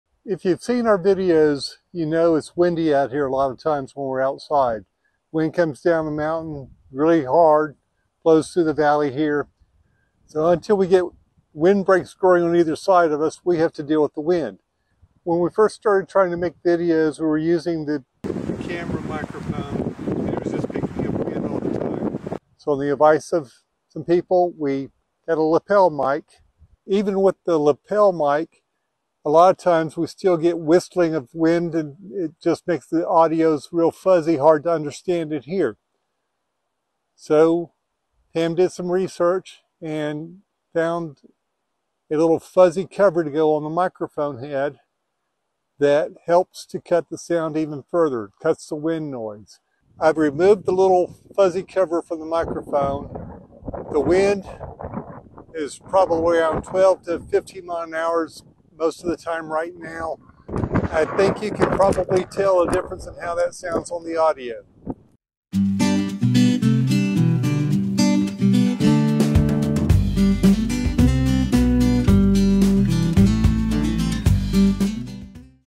Reducing Wind Noise Using a Microphone Windscreen--With Examples!
Using a windscreen on a microphone can improve audio by reducing the amount of wind noise heard on an audio track. This video gives examples of what the audio from a camera and a lapel mic sound like on a windy day, along with the dramatic improvement when a simple windscreen is put on the microphone. If you're recording audio in a windy environment, a windscreen on the mic is a must!

Link to the windscreen we use